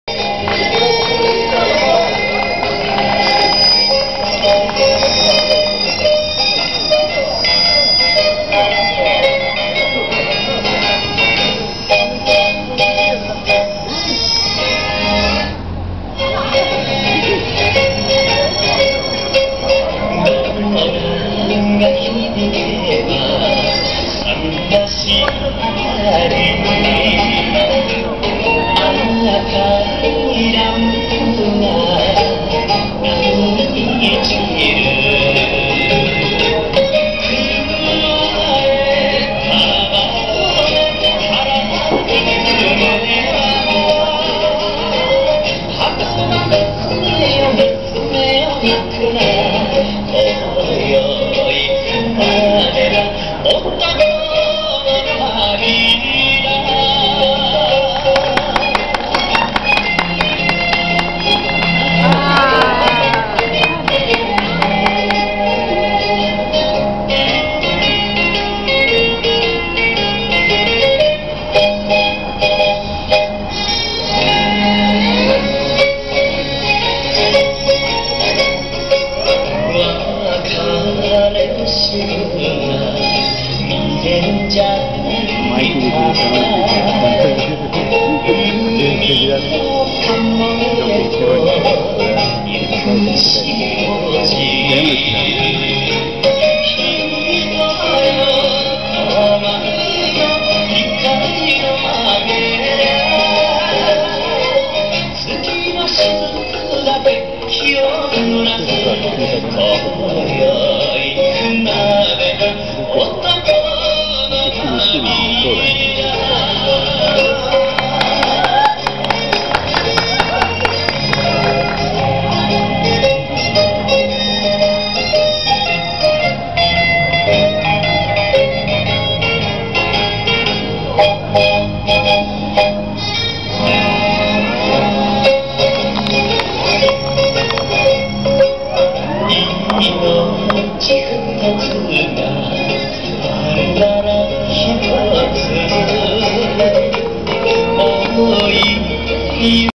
みなと三郎 歌謡 .ASF
十和田市の中心街に「まちなかひろば３０番」がオープン。
市民が無料で利用できる野外ステージです。
４月１８日はこけら落としで、大勢の市民がつめかけステージを楽しみました。